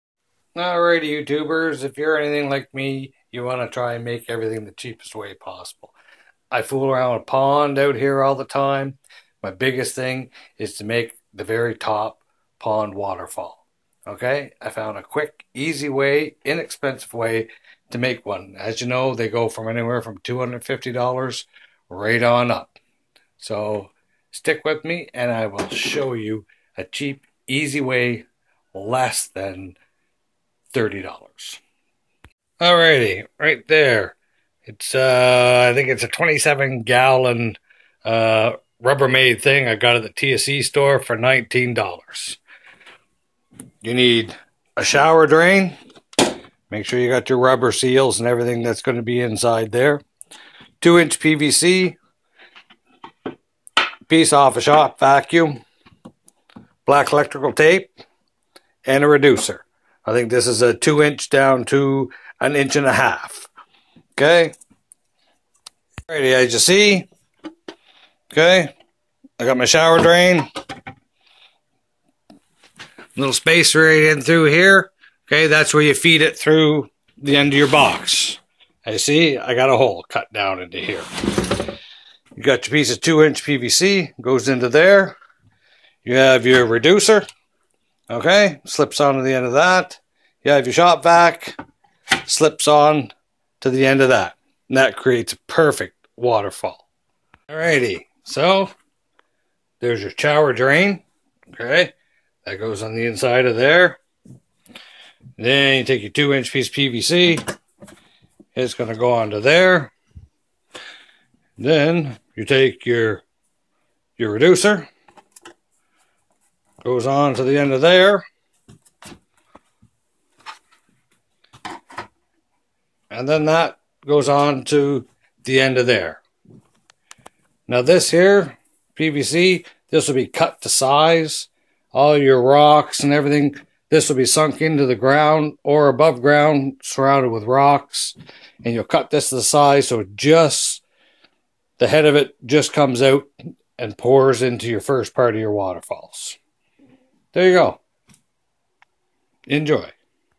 D I Y water fall box
cheap way to make a water fall box or spillway. by the way the tape is to take up the slop in the vac head and reducer